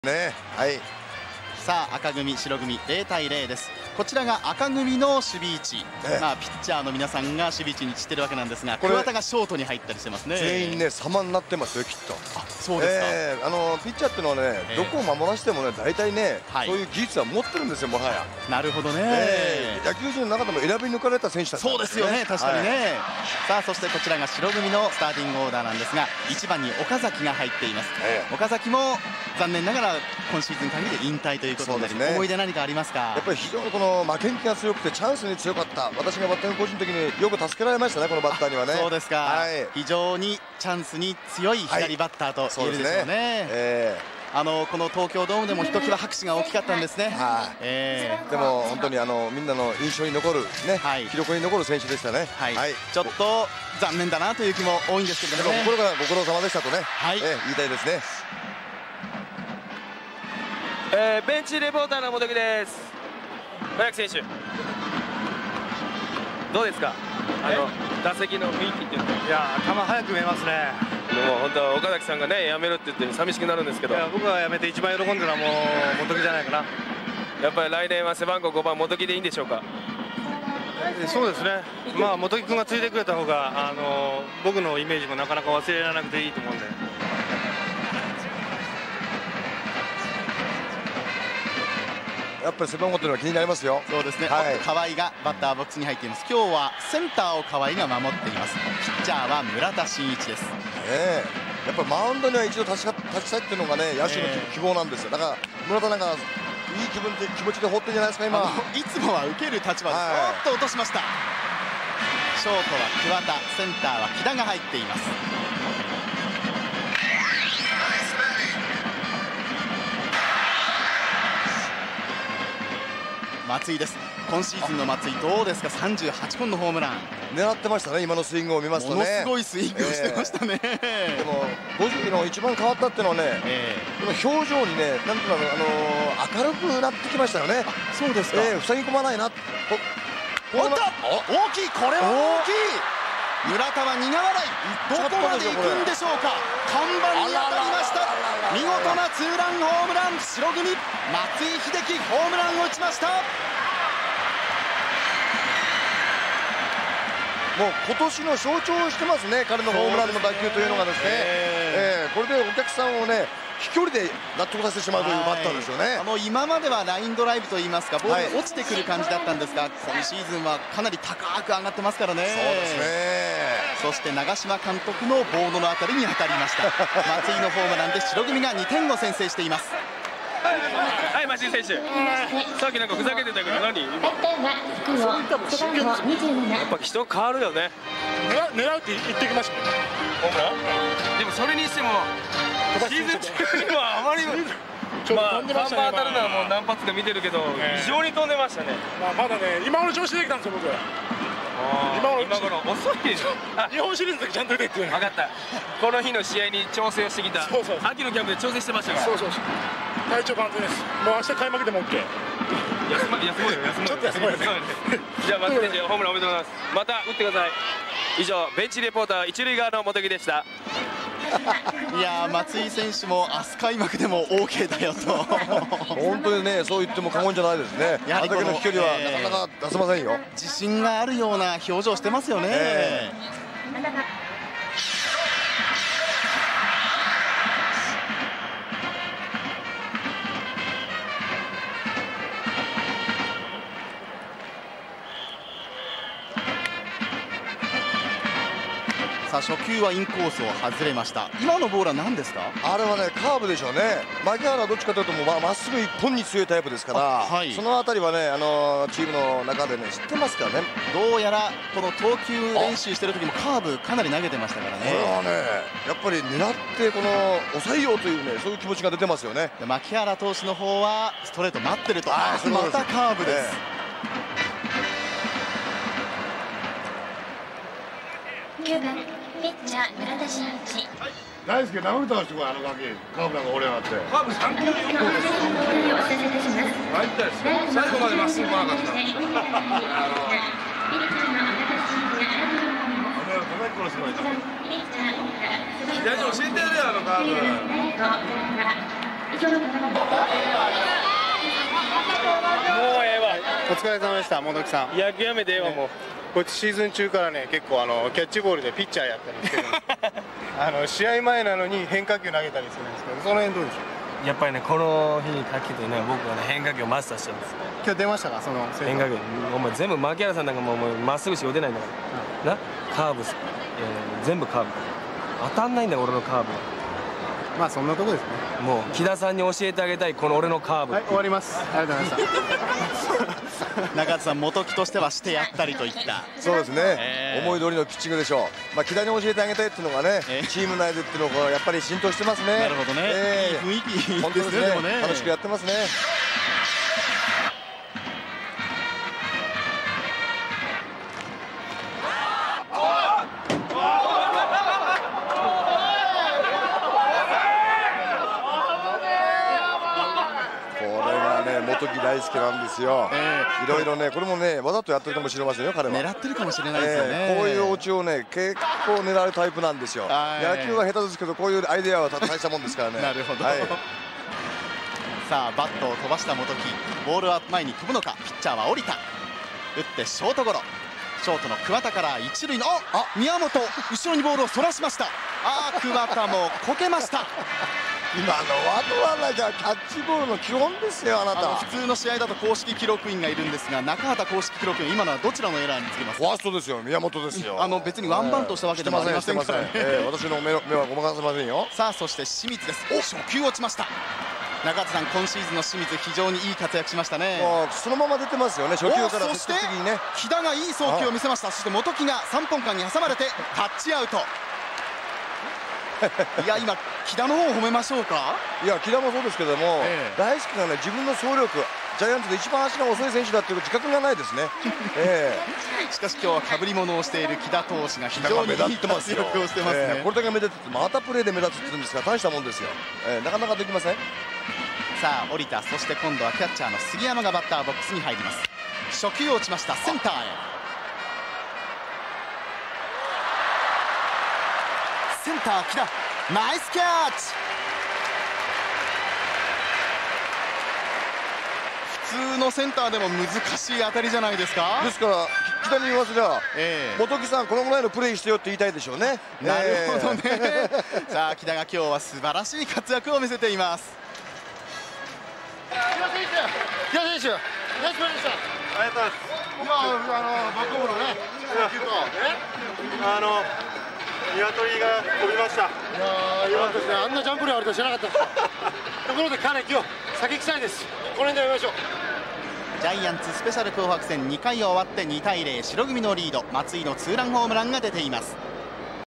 【セコム長嶋看板直撃】松井秀喜 元木大介のバットが敵ベンチ直撃 槇原寛己激怒 木田優夫大活躍
松井秀喜「明日開幕でもＯＫ」
村田真一 岡崎郁 川相昌弘 仁志敏久 織田淳哉 桑田真澄 清水隆行 堀内恒夫 中畑清

1996年ジャイアンツ・ファン感謝デー紅白戦 Part 2